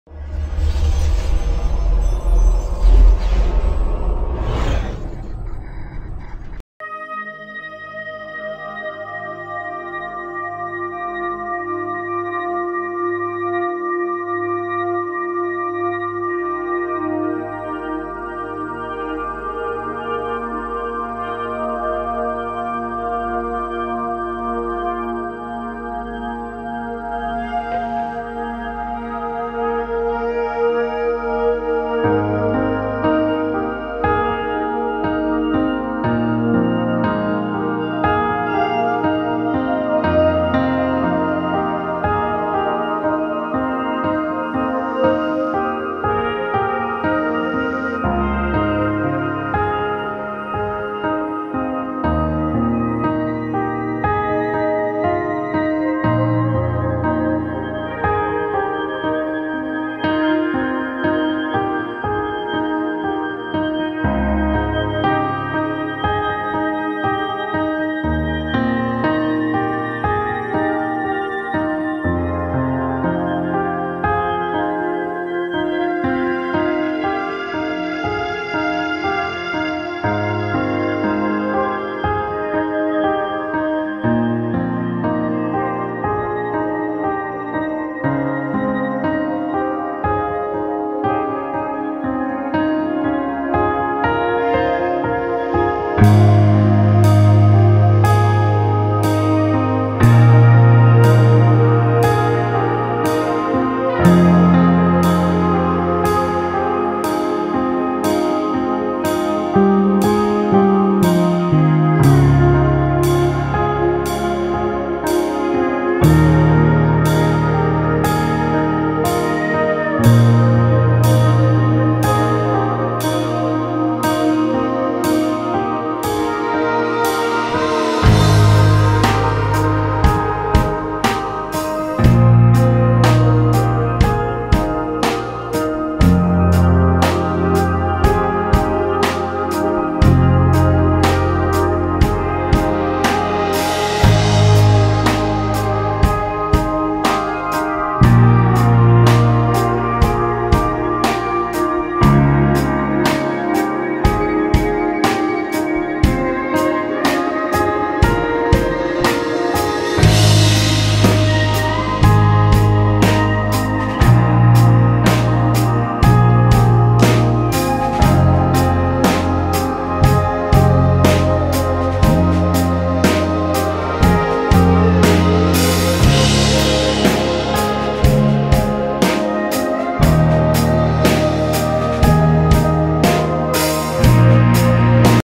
Aerial View of Lucknow 2015 with DJI Inspire1
Aerial View of Lucknow as a part of our Show reel during year 2015, Enjoy some breathtaking views of magnificent city of Nawab's.